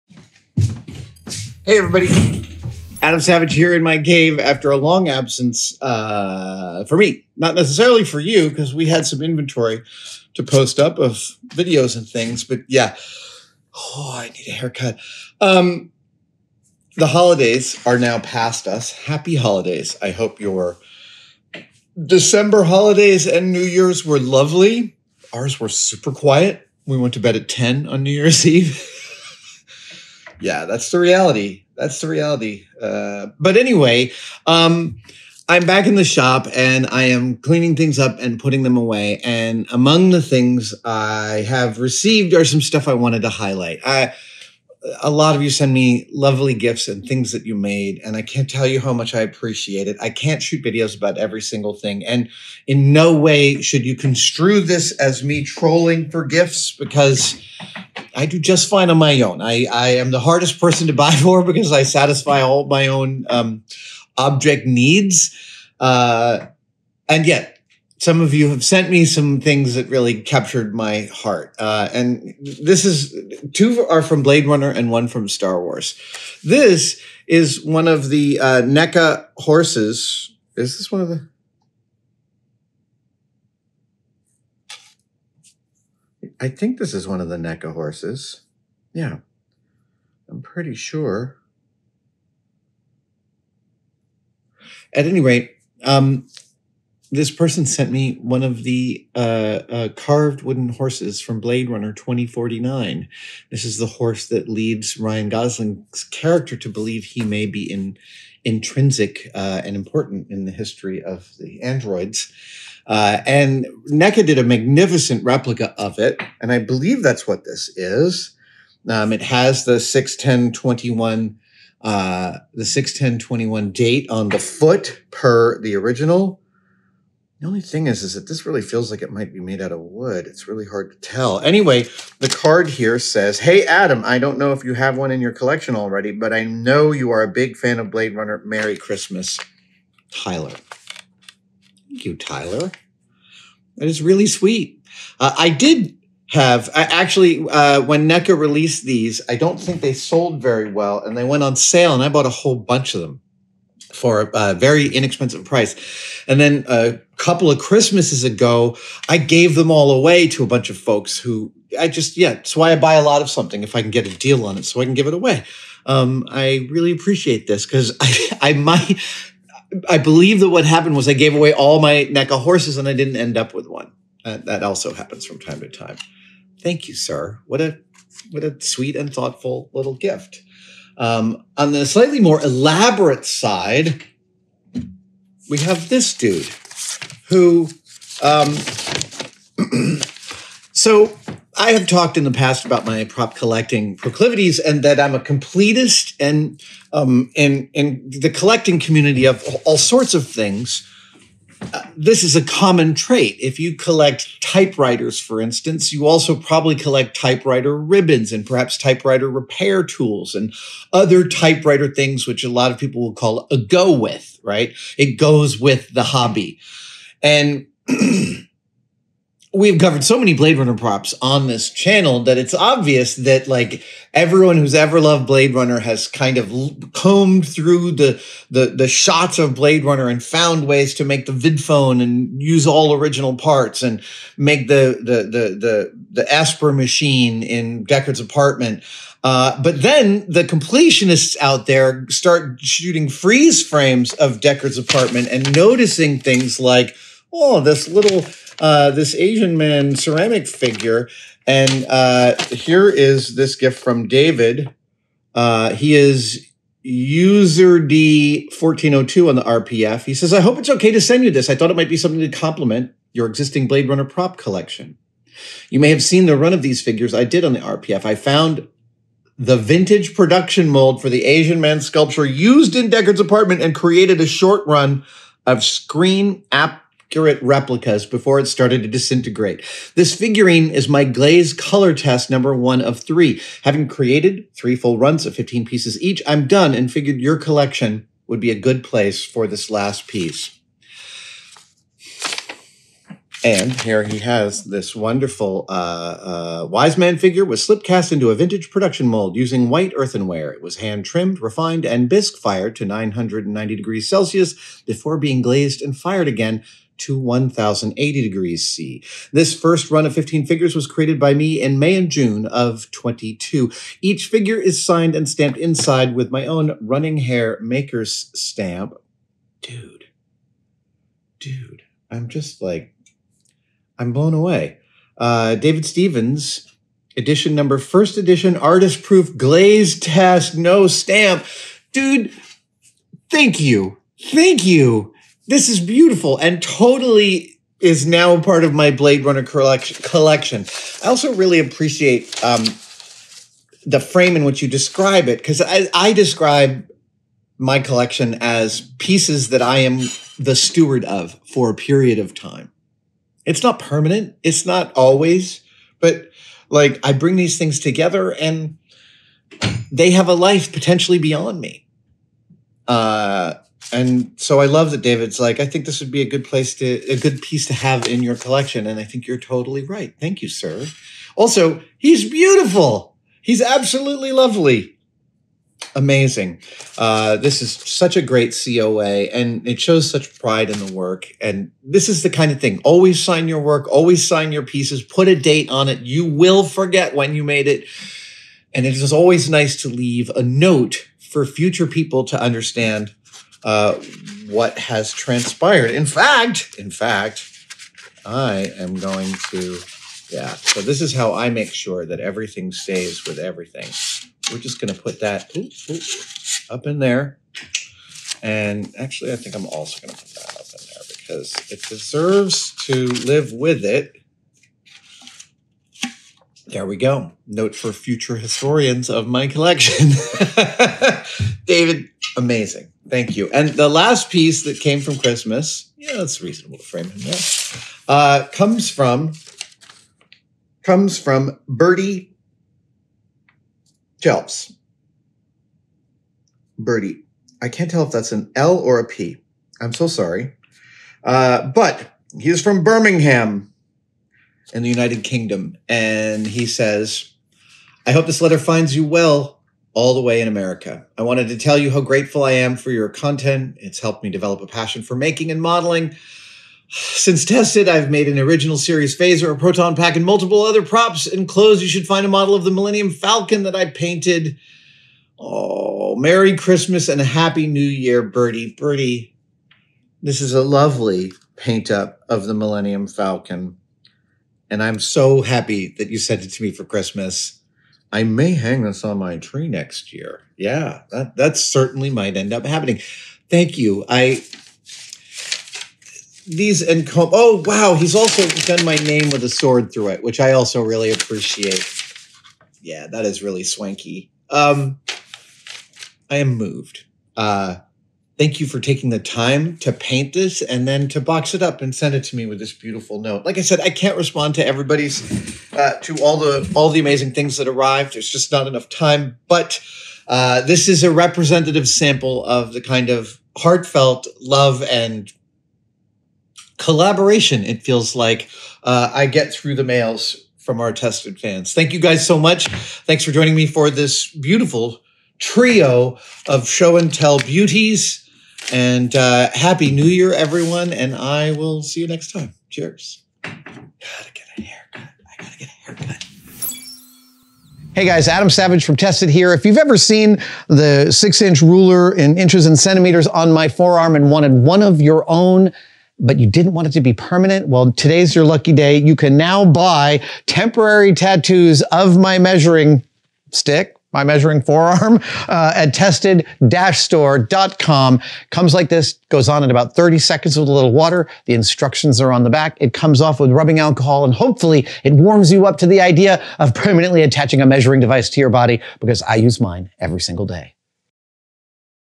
Adam Savage Opens His Christmas Mailbag Presents!
As Adam returns back to the cave after the holiday break, he opens a few of the mailbag gifts received from Tested viewers, including two Blade Runner-themed objects! Thanks to everyone who sent in a mailbag gift--they are all very much appreciated!

Tested and Adam Savage Ts, stickers, (de) merit badges and more